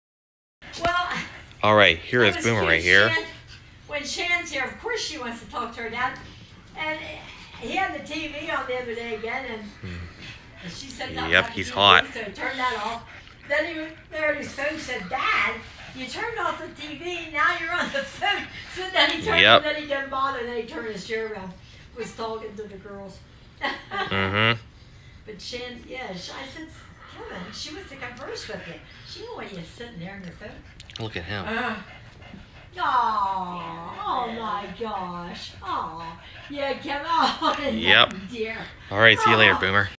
Boomer at My Grandma’s House - 7/5/19
Here is a video of my grandma’s Jack Russell terrier dog Boomer at her house on Friday, July 5, 2019. Enjoy!